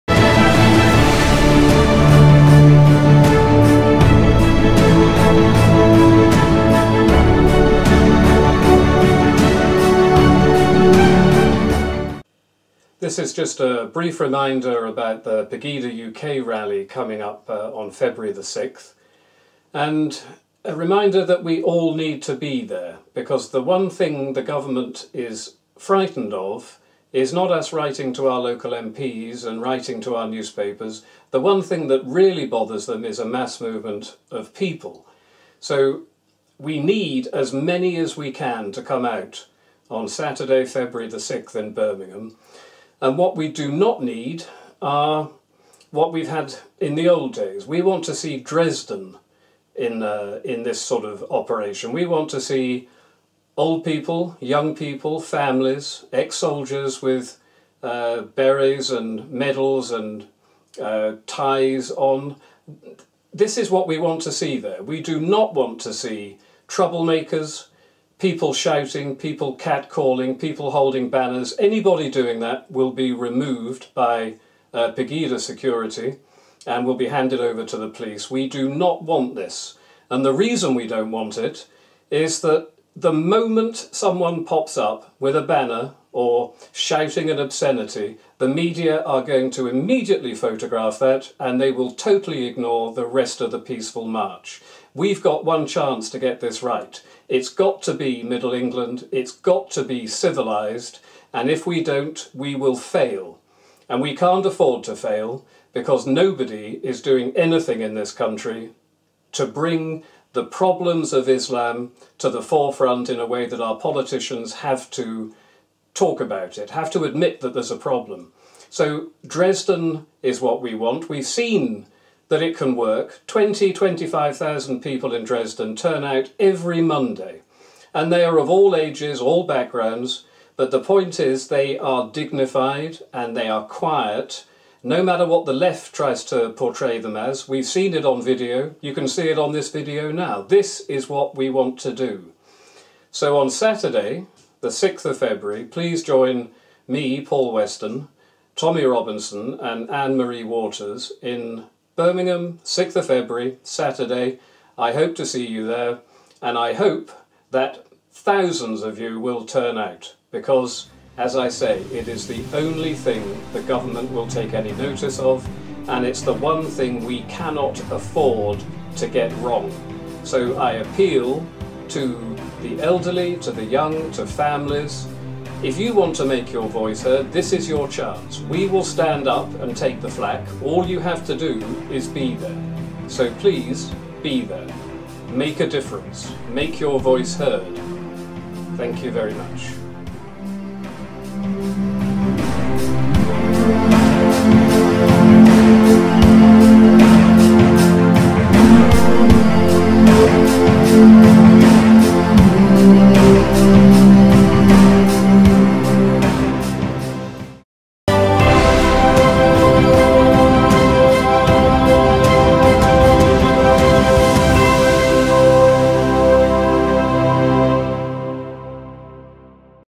Paul Weston's reminder to come to Birmingham
Join PEGIDA UK in Birmingham on 6 February 2016